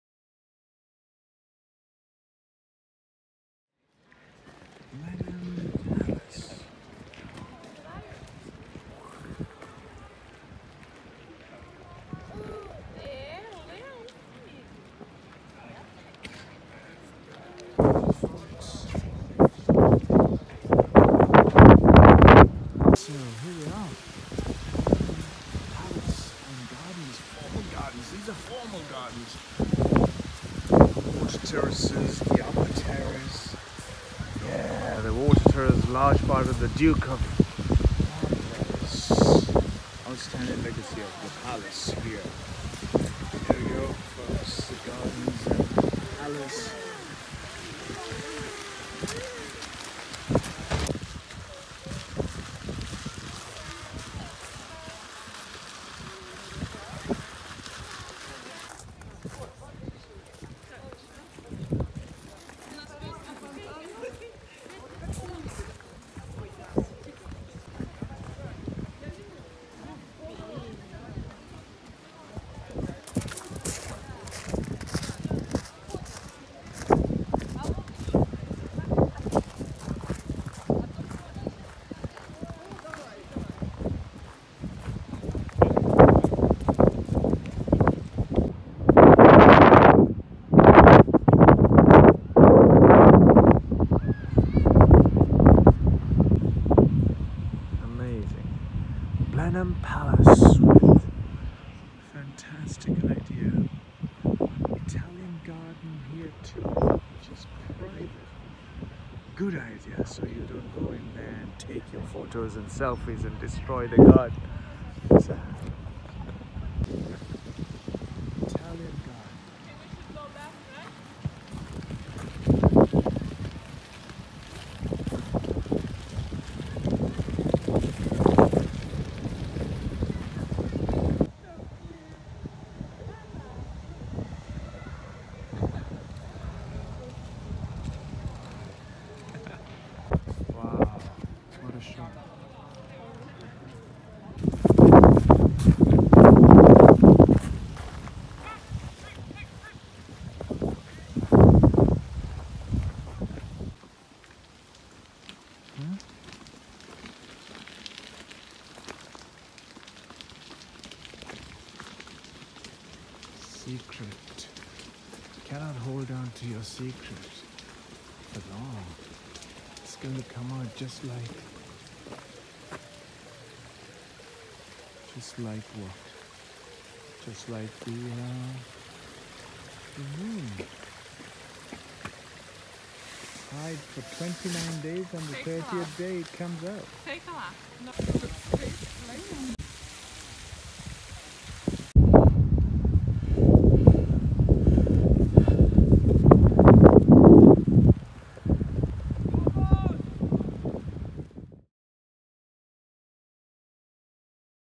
Blenheim Palace and Gardens
Blenheim Palace a World Heritage Site with over 300 years of History and home to the 12th Duke of Marlborough and his Family.